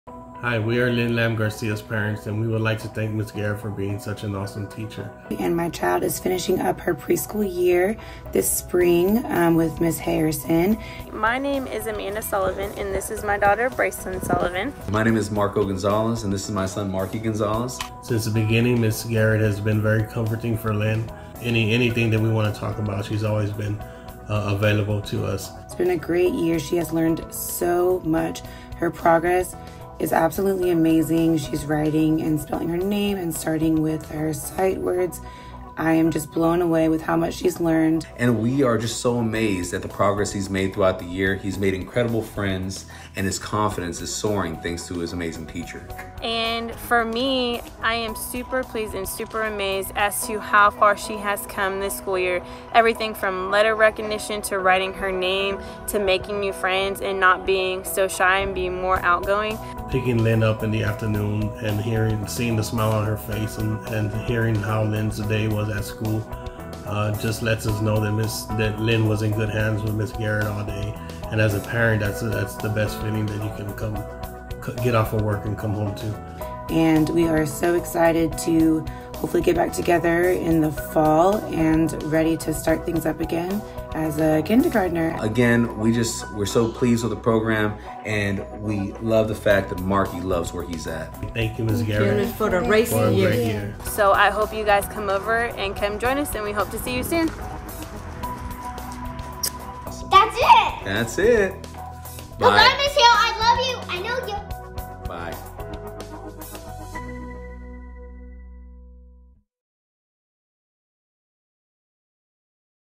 A Few Words From Our Spring ISD Pre-K Parents!
Our parents talk about their great experiences with Spring ISD Pre-K!